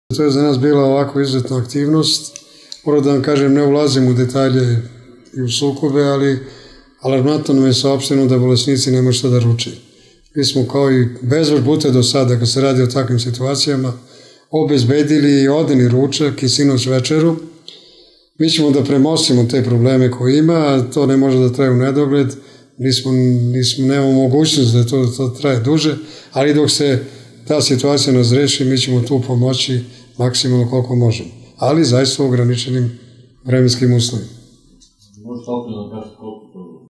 Premostićemo nastalu situaciju ali ne više od par dana, 05.02.2014.
Požarevačka organizacija Crvenog krsta Požarevac, juče je na zahtev gradonačelnka grada Požarevca počela da distribuira hranu za bolesnike koji se nalaze na lečenju  u opštoj bolnici u Požarevcu a posle zabrane direktora Bosića da se koristi hrana iz kuhinje koja je do tada spremala hranu.